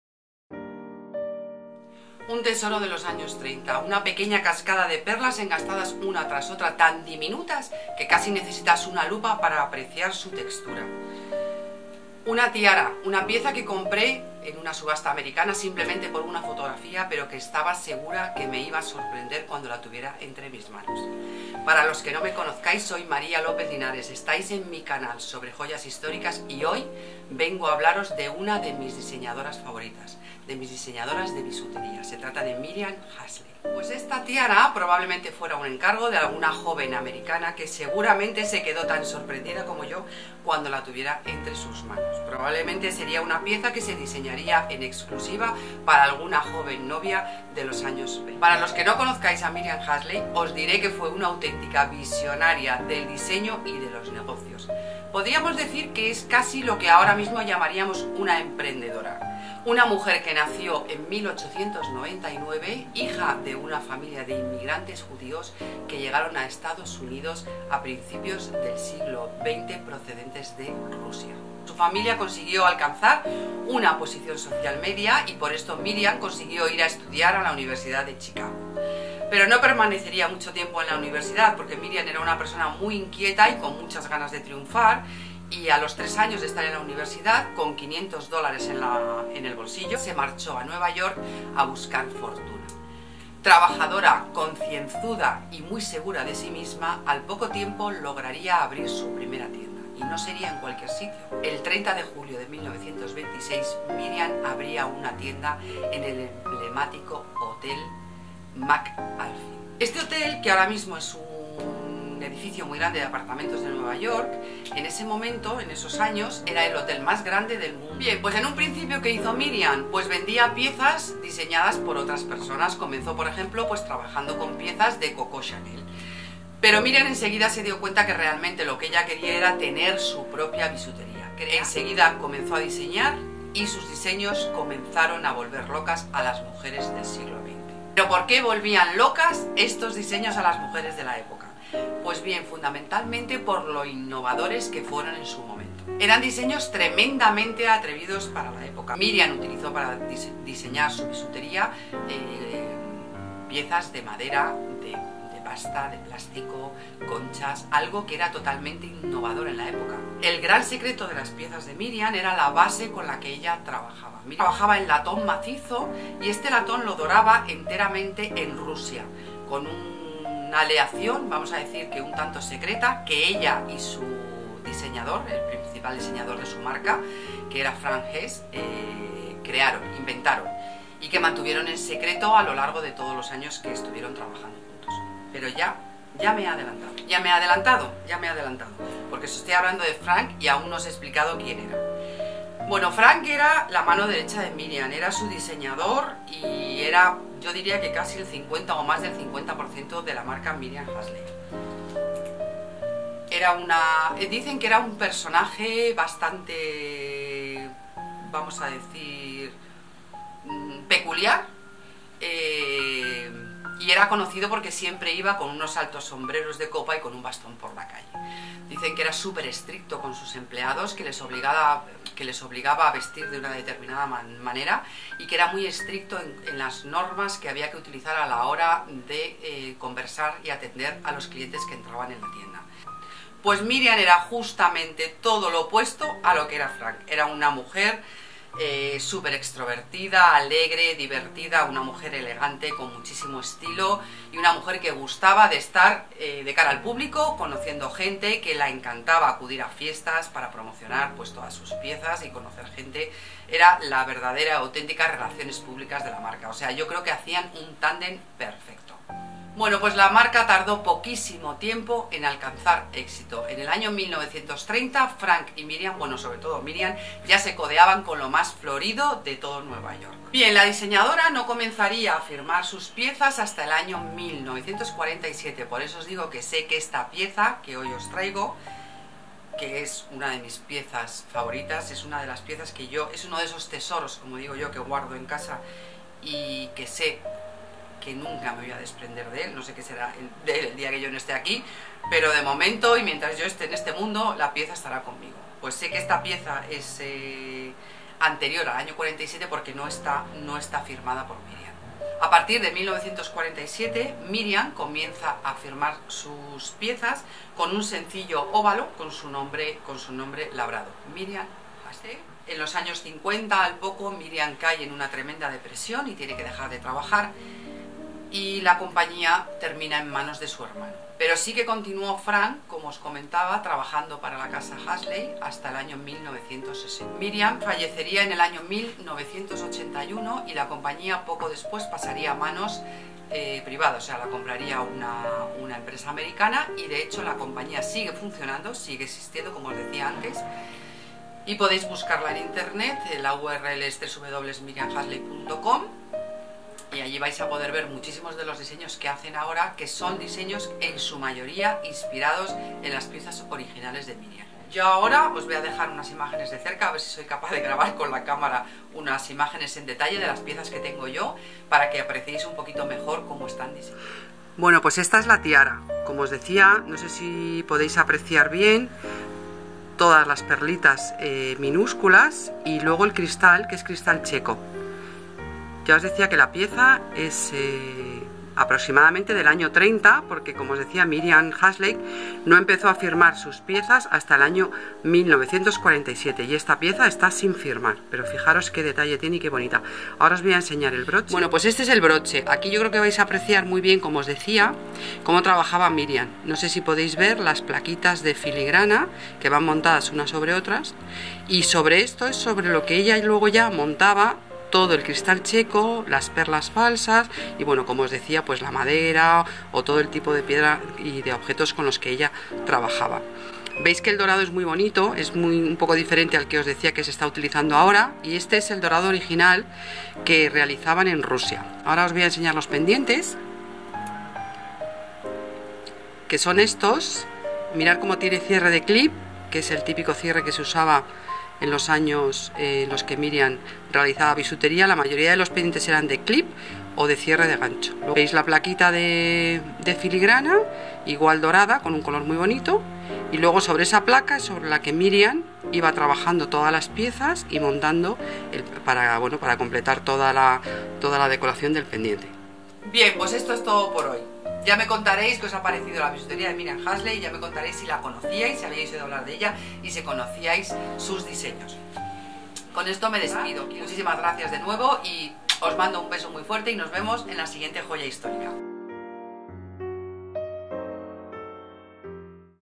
Miriam Haskell - La historia de una gran emprendedora
Este es el último video que grabé en nuestro emblemático espacio de la calle de Claudio Coello, 60. Fue justo unos días antes de marcharme de vacaciones y de cerrar para siempre.

Pero ya sabéis que ahora tengo una preciosa tienda online, en la que paso la mayor parte de mi tiempo y que, la verdad, me está dando muchísimas satisfacciones.

Mientras editaba el video, me ha entrado cierta nostalgia al recordar la cantidad de gratos momentos que he pasado durante los casi 30 años entre esas paredes, pero ya sabéis que los tiempos cambian y que hay que saber adaptarse y aprovechar las nuevas oportunidades que nos llegan en la vida, o que nosotros mismos debemos provocar.

Bueno, no me enrollo más… Este mes he venido a hablaros de una de mis diseñadoras de bisutería favoritas, se trata de Miriam Haskell.

Me chifla su trabajo, y la verdad es que he disfrutado muchísimo metiéndome en su vida para conocerla mejor y poder contárosla a vosotros. Lo he disfrutado de lo lindo.

Os dejo el video con toda su historia, y al final os enseño algunas de las piezas que tengo suyas en mi pequeña colección de bisutería antigua.

¡Ya me contareis qué os ha parecido su estilo y si os ha gustado su trabajo!